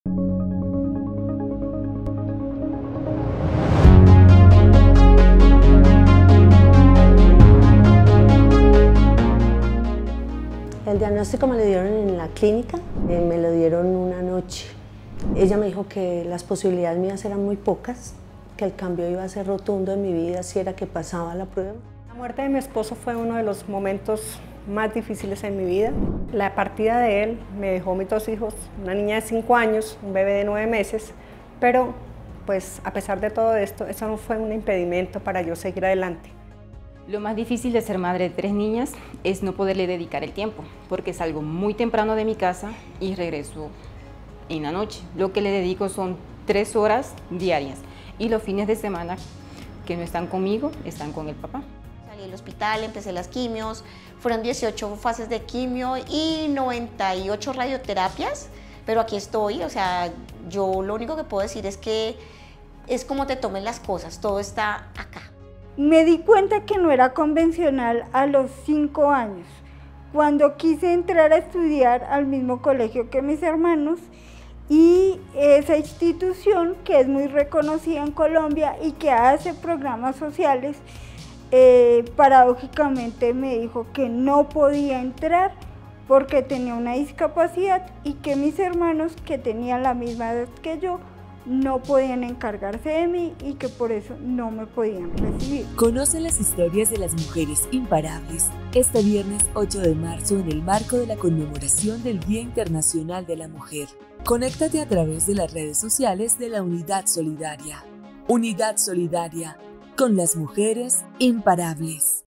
La Asociatividad Solidaria es con las Mujeres
Las mujeres colombianas son aguerridas, luchadoras, tenaces, inteligentes, trabajadoras, protectoras y solidarias. Para ellas un homenaje en el mes de la mujer.